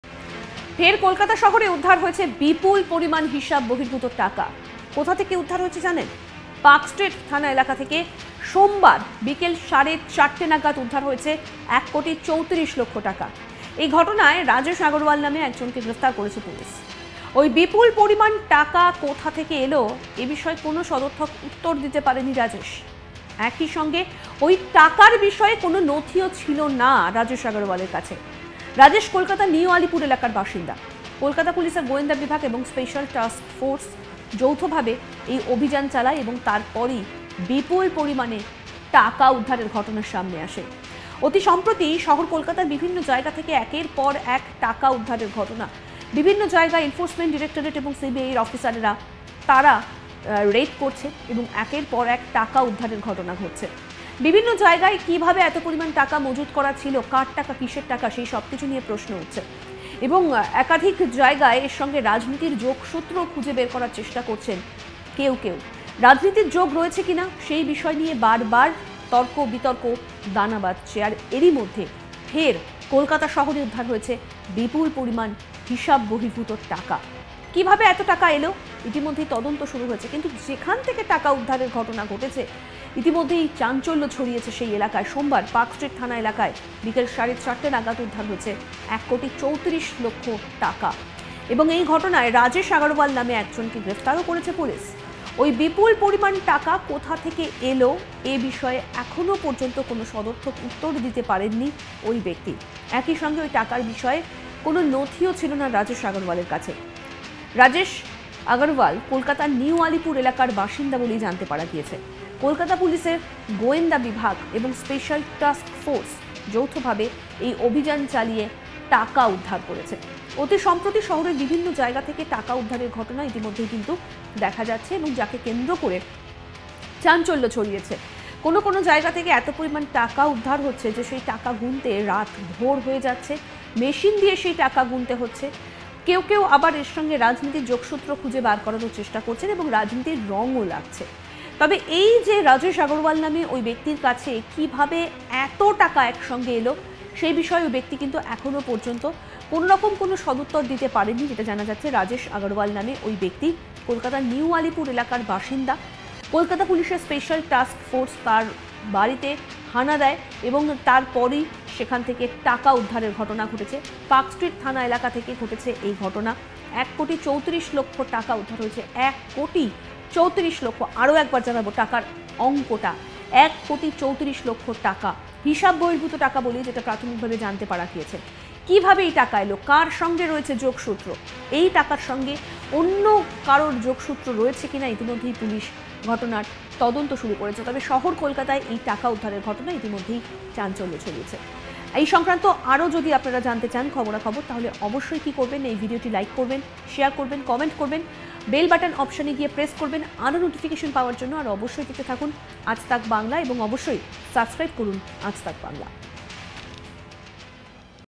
কলকাতার পার্ক স্ট্রিটে ফের মিলল টাকার পাহাড় | Money recover in Kolkata | Aaj Tak Bangla
কলকাতার পার্ক স্ট্রিটে ফের মিলল টাকার পাহাড় | Mone Recover in Kolkata | Aaj Tak Bangla